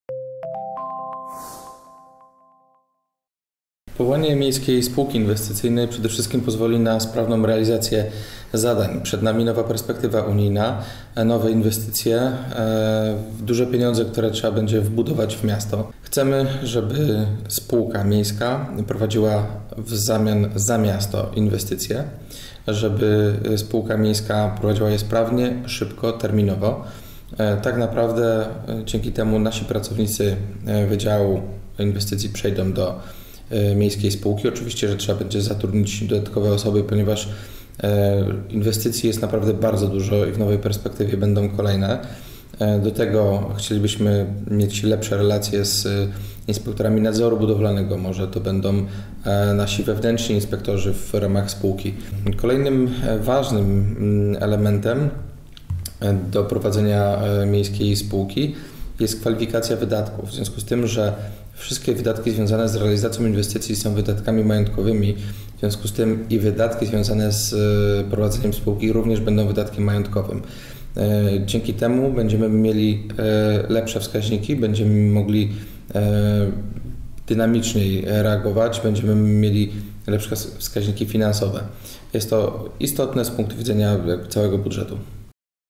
Miejska Spółka Inwestycyjna
Czy w Gorzowie powstanie miejska spółka inwestycyjna? Urzędnicy mają taki plan. Powodem jest odpływ urzędników z miejskiego wydziału inwestycji. Odchodzą między innymi z powodu niskich płac. Miejska spółka ma szybko, sprawnie i terminowo realizować zadania inwestycyjne. W nowej perspektywie finansowej będzie ich jeszcze więcej.